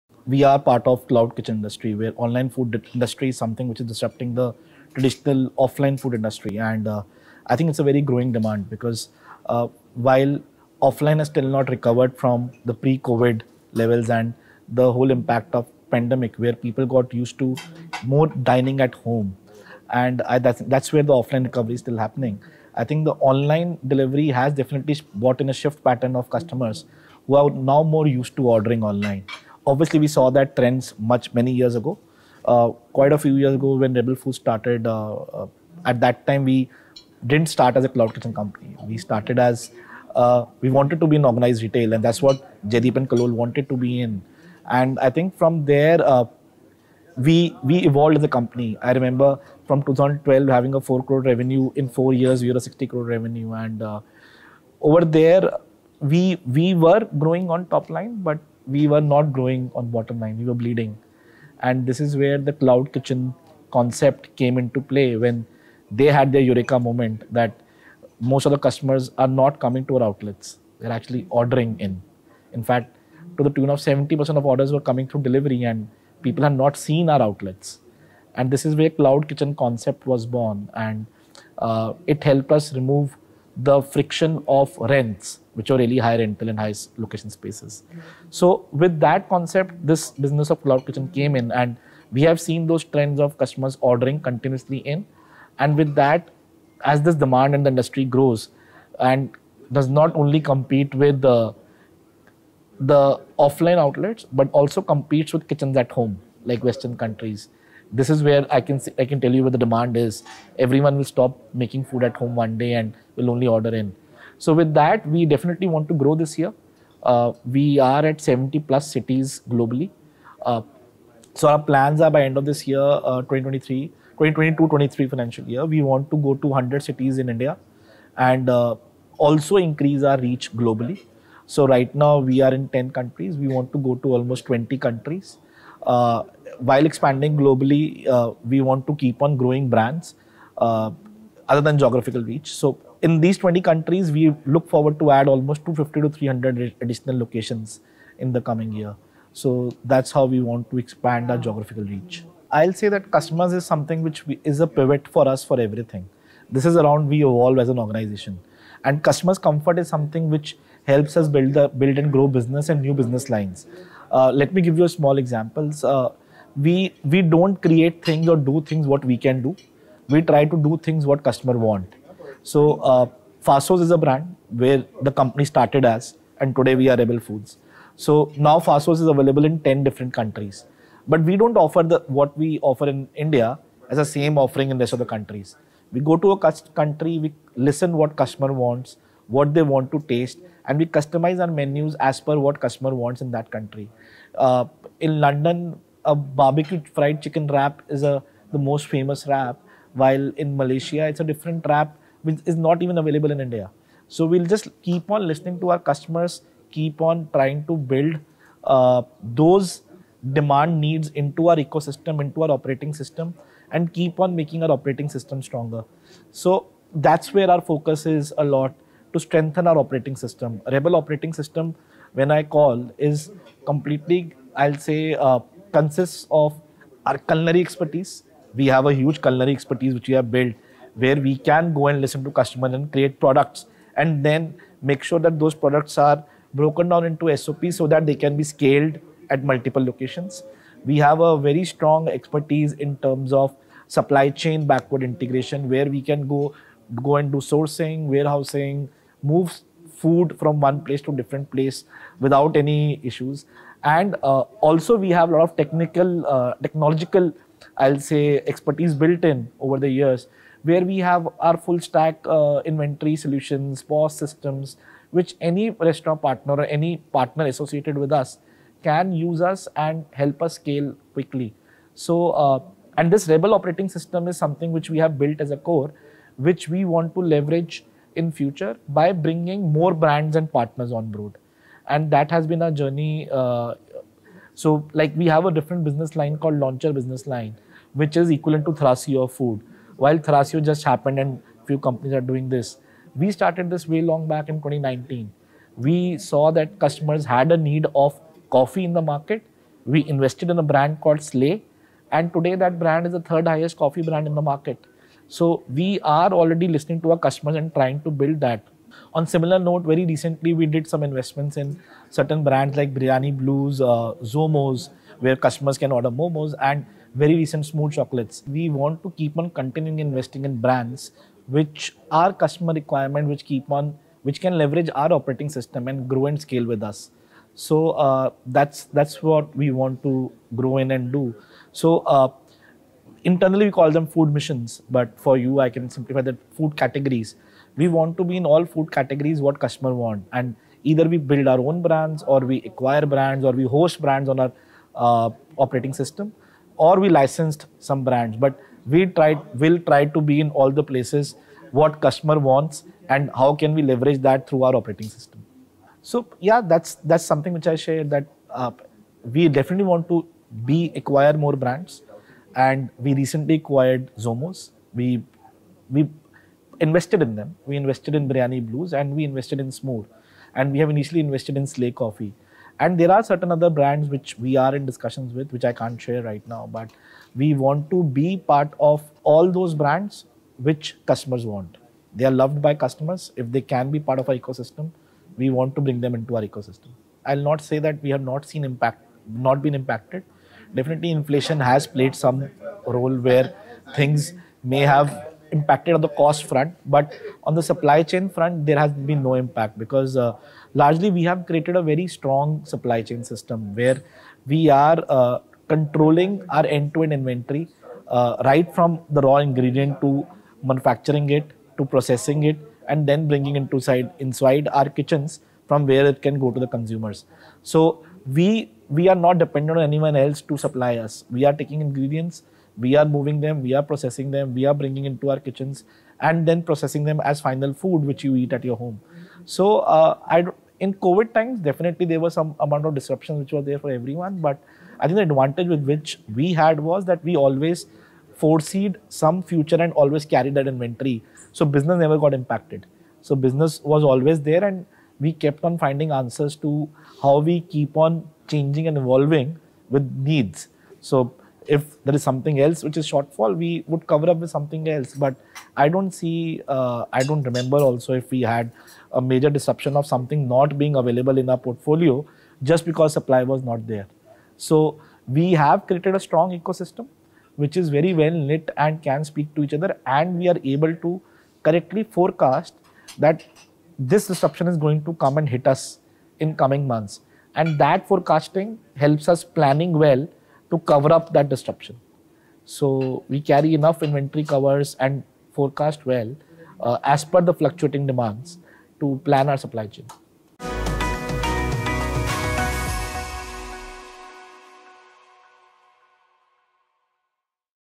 We're planning to expand across 100 cities by end of FY 2022-23, says Karan Singla of Rebel Foods
Karan Singla, VP and Head of operations, India Business, Rebel Foods shares his global expansion plans with ETRetail. The company already has its presence in 10 countries and aims to expand to 20 countries. Singla also shares the investment plans for the brand.